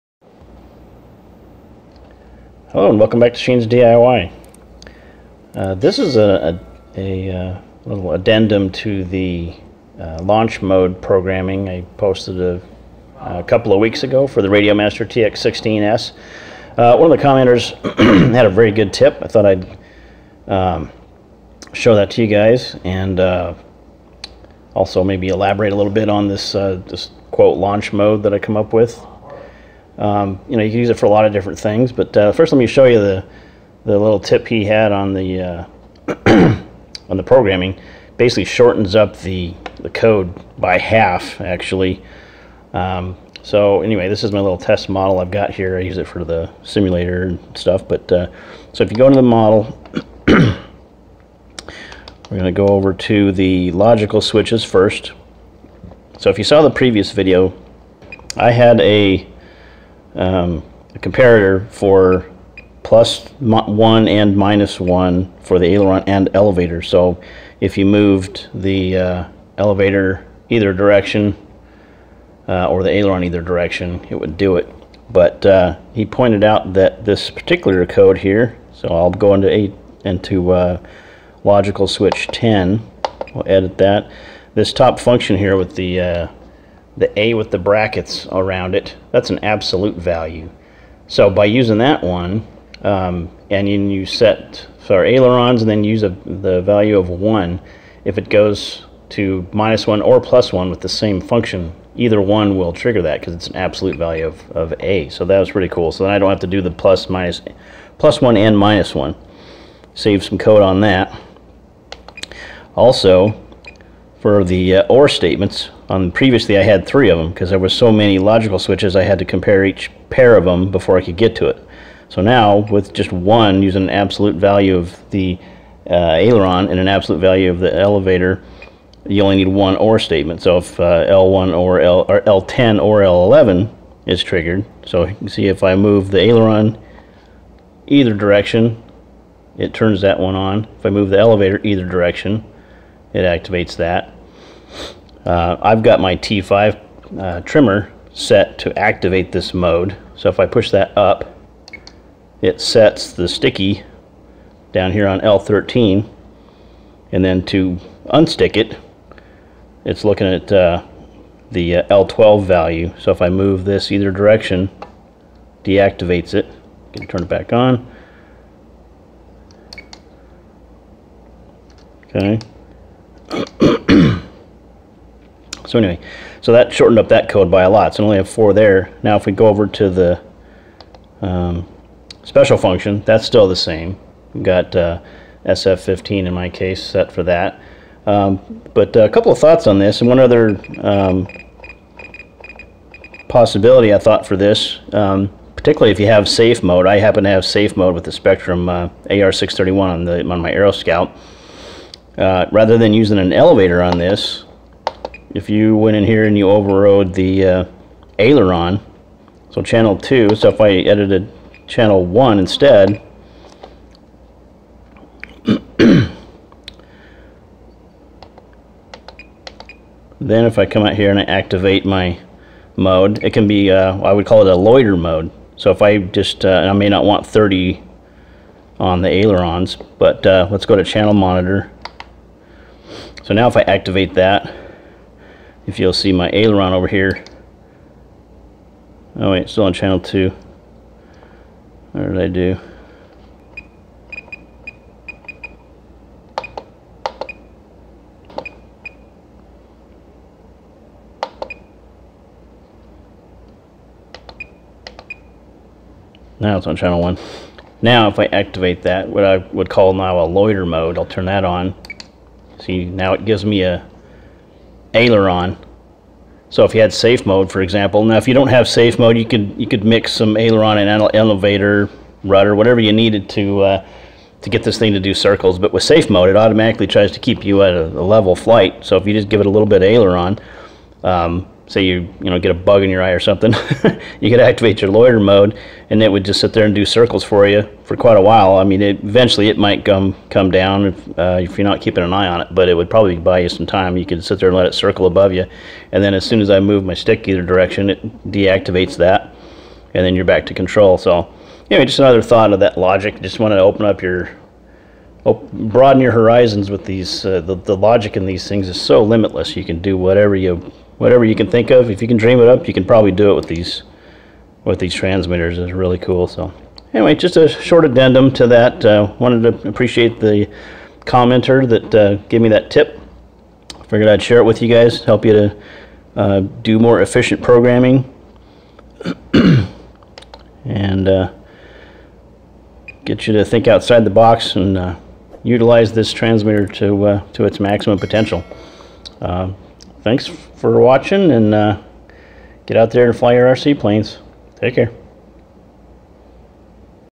RC RadioMaster TX16S Aircraft Launch/Loiter Mode Programming Update
*Check for a newer update of the "Launch/Loiter" mode!  In testing I've found a flaw in these first 2 versions of the logic code that might prevent the mode from exiting when you move the stick!  I'll post a link to the update once I upload it (after 12/3/2022).

A commenter suggested a way to reduce some logic on the "launch mode" we discussed a couple videos ago so lets try it out!

This control logic uses Logic Switches and a Special Function to produce a basic "Launch Mode" to allow hand launching of fixed wing RC aircraft. You could also use this mode for a loiter mode to let the aircraft circle above for a while.  Any other ideas to use this "auto pilot" mode for post in the comments below.

The TX16S transmitter and EdgeTX / OpenTX are a game changer!

Interested in the Radiomaster TX16S?  Here is an Amazon link for the one I purchased that will help the channel.  Thanks!